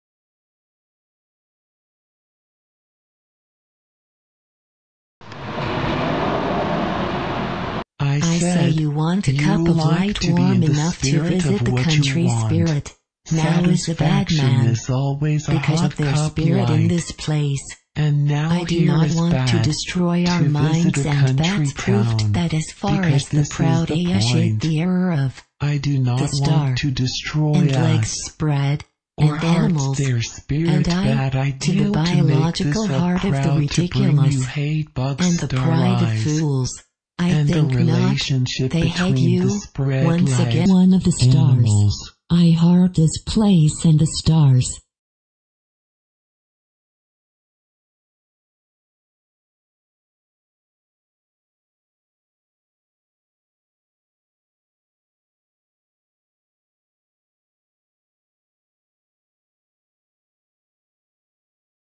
YOU/I AND STARS (realistic moth film dubbed very realistic)
beach moth

camera and music and written by joe bussiere
starring beach moth and a leaf and a candle

on facebook and tumbler too